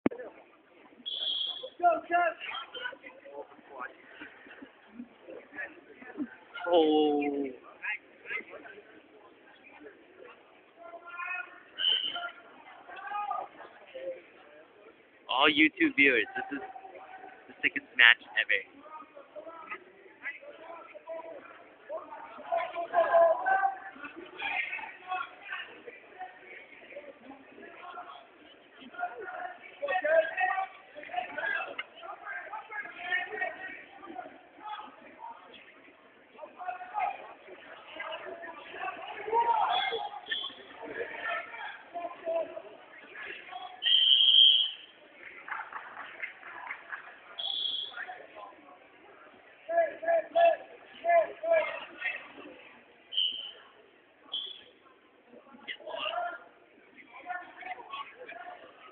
07 Weymouth Tournament pt.1
130 weight class championship Josiah Quincy(red)vs. Quincy(blue)